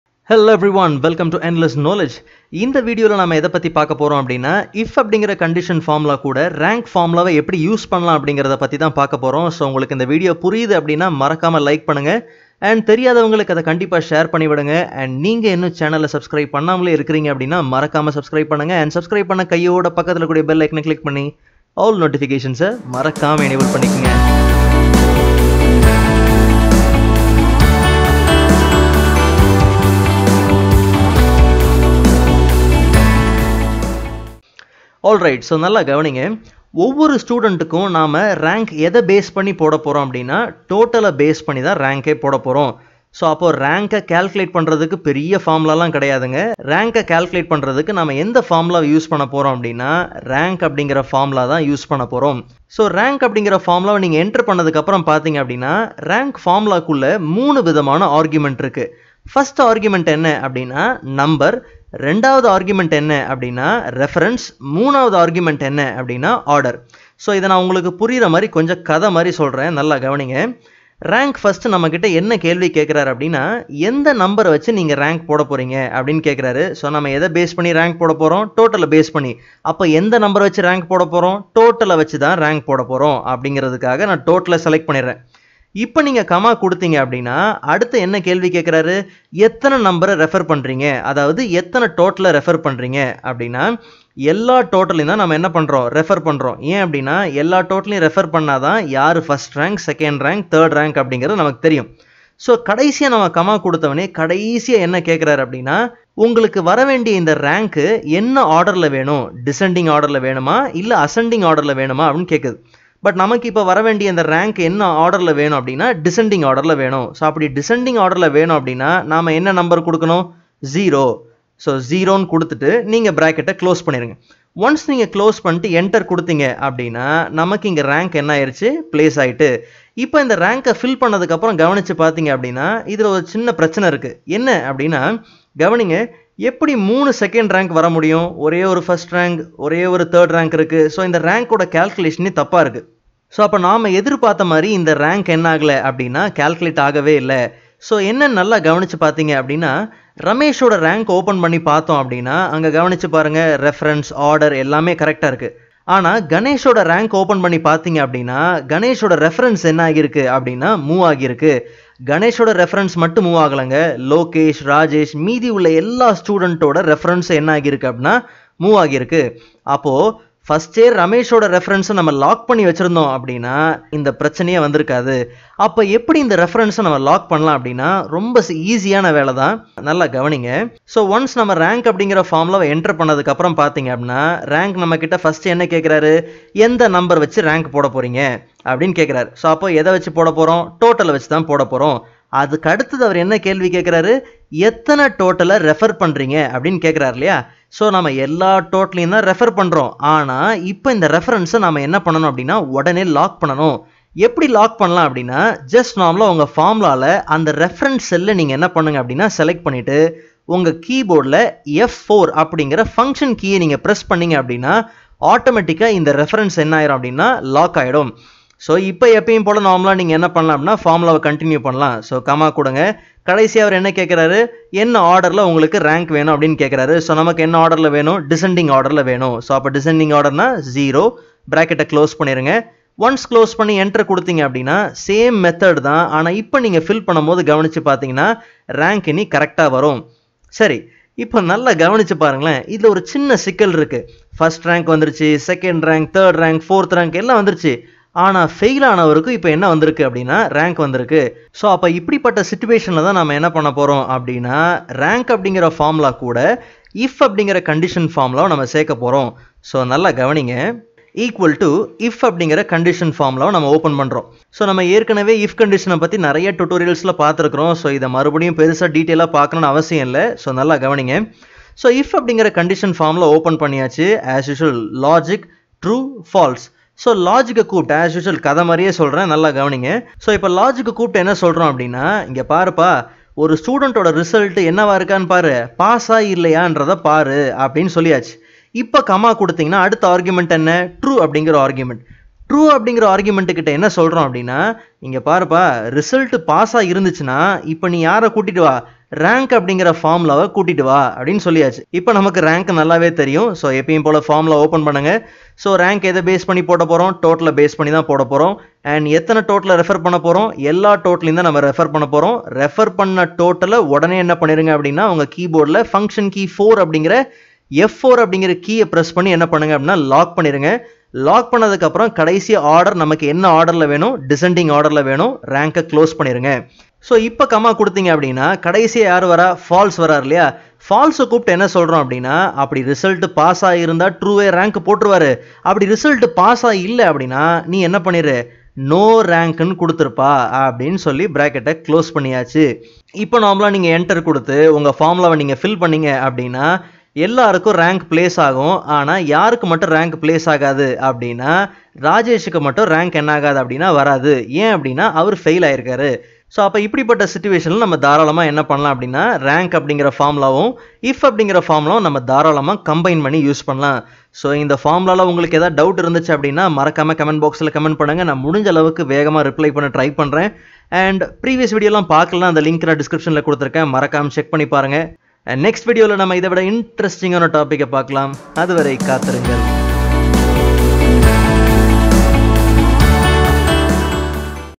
RANK Formula in Excel in Tamil
In this tutorial, learn how to use Rank formula with IF Condition step by step.